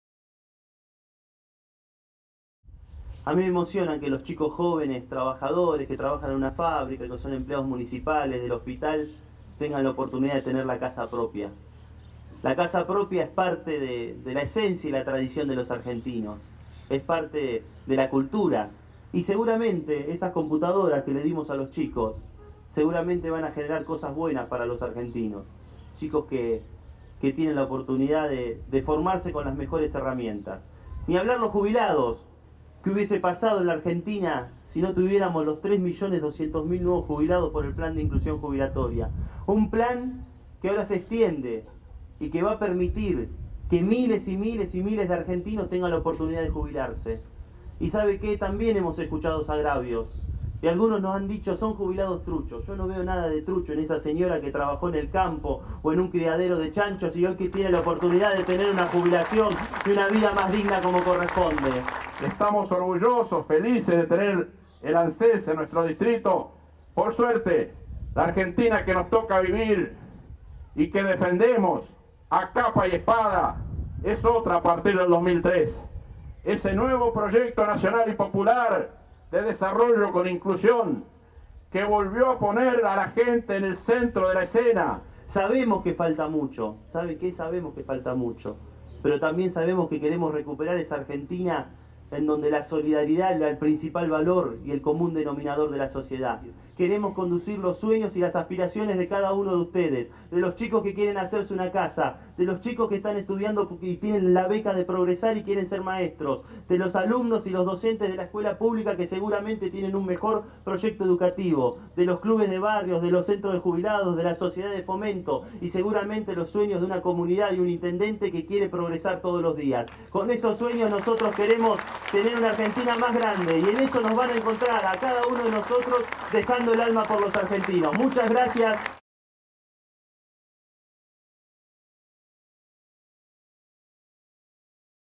Diego Bossio inauguró oficina de ANSES en Vedia 19-09-2014
El Director Ejecutivo de la ANSES, Diego Bossio, visitó el partido de Leandro N. Alem, donde inauguró la oficina del organismo en Vedia, provincia de Buenos Aires. Allí estuvo acompañado por el intendente local, Alberto Conocchiari, y el diputado nacional, Oscar Romero.

Luego de recorrer las instalaciones, Bossio sostuvo: “Me emociona que las personas puedan acceder a la casa propia. Eso es parte de nuestra historia y cultura. También me alegra entregar computadoras del programa Conectar Igualdad y que haya nuevos jubilados en el país. Nosotros sabemos cómo queremos vivir en la Argentina: en comunidad, unidos y trabajando”.

Conocchiari, a su turno, expresó: “La Argentina es otra desde 2003. Se forjó un nuevo proyecto de desarrollo con inclusión y trabajo concreto. También quiero agradecerle a Bossio por estar acá. Él privilegia el contacto con la gente, tenemos esperanza en su juventud y gestión”.

La delegación de la ANSES estará ubicada en Diagonal Estrada 65, contará con cómodas instalaciones para la atención al público, funcionará de lunes a viernes de 8 a 14 horas y dependerá de la delegación ubicada en Junín. De esta manera, los vecinos de Vedia, Colonia Alberdi, Fortunacha y El Dorado, entre otros, tendrán a su disposición una oficina de atención del organismo previsional cercano a sus domicilios.

Por otra parte, Bossio firmó un convenio con el intendente para la cesión de lotes. De esta manera, se incrementará la oferta de terrenos y los vecinos tendrán más posibilidades para construir sus viviendas PRO.CRE.AR.

Posteriormente, el titular de la ANSES entregó tarjetas ARGENTA a jubilados y pensionados, tarjetas de débito PROGRESAR a jóvenes que acreditaron su escolaridad, certificados de crédito PRO.CRE.AR. a familias locales, netbooks de Conectar Igualdad a los alumnos y docentes de la Escuela de Educación Media Nº 3 “Julio Argentino Roca” y de la Escuela de Educación Secundaria Nº 5, y banderas de ceremonia a centros de jubilados locales.

La Seguridad Social en Leandro N. Alem

Acerca de Conectar Igualdad, los docentes y alumnos de Leandro N. Alem ya recibieron 2503 equipos del plan de inclusión digital repartidos en 12 establecimientos educativos.

Con respecto a PRO.CRE.AR., se encuentran en marcha 58 viviendas, por un monto de créditos solicitados de $9,3 millones. Además, en el último sorteo efectuado el pasado 10 de junio, se inscribieron 206 personas, de las cuales resultaron sorteadas 171, que están tramitando su crédito.

La nueva moratoria en marcha en la provincia

En solo una semana, 80.915 bonaerenses ya solicitaron un turno para poder acceder a la nueva moratoria previsional. De esta forma, la provincia lidera las estadísticas en el ámbito nacional. También se ubica entre las provincias con mayor uso intensivo de las tecnologías, accediendo a los derechos sociales de la ANSES através de la web

A partir de la entrada en vigencia de la nueva moratoria previsional, 500 mil personas tendrán la posibilidad de regularizar sus aportes no realizados en el período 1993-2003, en 60 cuotas, y acceder a la jubilación. Gracias a esta medida, la cobertura previsional aumentará 6,2%, y pasará del actual 93% a abarcar a casi la totalidad de los adultos mayores de nuestro país.

Al respecto, Bossio expresó: “Nuestra Presidenta tomó una firme decisión al incorporar a cientos de miles de personas en el sistema previsional argentino en esta segunda etapa de la moratoria previsional. Los adultos mayores siempre fueron una prioridad para este Gobierno, y estas políticas inclusivas así lo confirman. Además, el trámite es gratuito y sencillo, y cuentan con variadas opciones para solicitar su turno. Recordamos que no es necesario pagar a gestorías para acceder a la jubilación, ya que en la ANSES el asesoramiento es gratuito, fácil y rápido. Una vez que la persona paga la primera cuota, en el mes siguiente ya tendrá liquidada su jubilación, por lo que ni siquiera se acumulan retroactivos. Queremos que todos los bonaerenses que puedan acogerse a la moratoria puedan hacerlo con facilidad y sin tener que pagar nada”.

La nueva Ley permitirá mejorar la sustentabilidad del sistema gracias al incremento en la recaudación y, además, generará crecimiento, porque las nuevas jubilaciones se volcarán al consumo y generarán un ciclo virtuoso en la economía, mejorando también la distribución del ingreso.